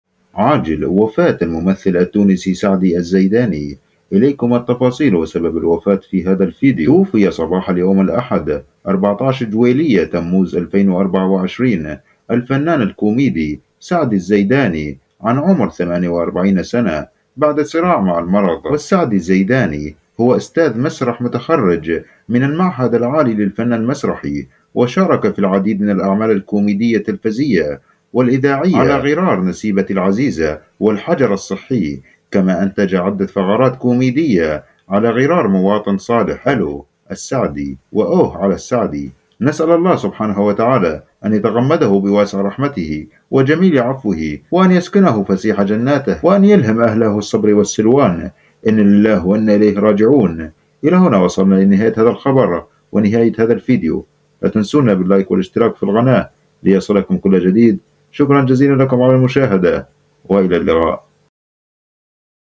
عاجل سبب ولحضة وفاة الممثل التونسي سعدي الزيداني , الفنان سعدي الزيداني _ Saadi Al-Zaidani
اليوم سنتعرف على تفاصيل وسبب وفاة الفنان  الممثل الكوميدي التونسي سعدي الزيداني _ Saadi Al-Zaidani